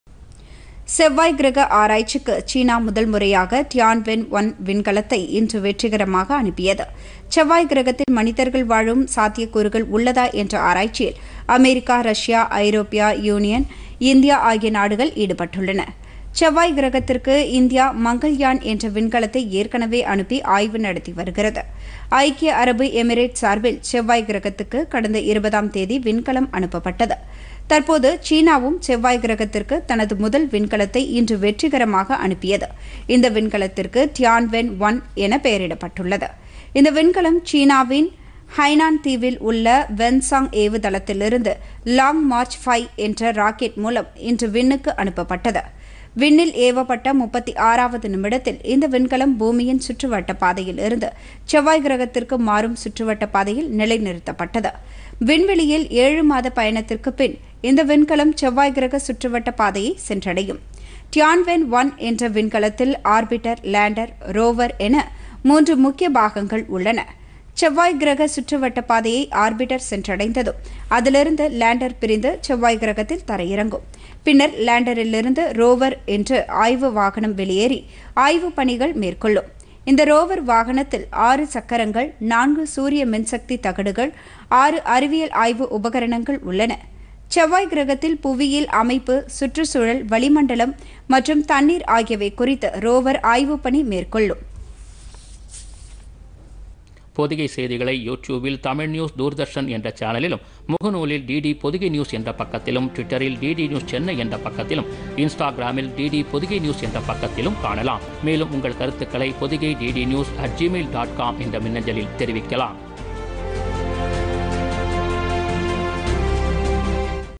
செவ்வாய் கிரகத்திற்கு விண்கலத்தை சீனா செலுத்தியது
முதல்முறையாக செவ்வாய் கிரக ஆராய்ச்சிக்கு  Tianwen- 1 விண்கலத்தை சீனா வெற்றிகரமாக செலுத்தியது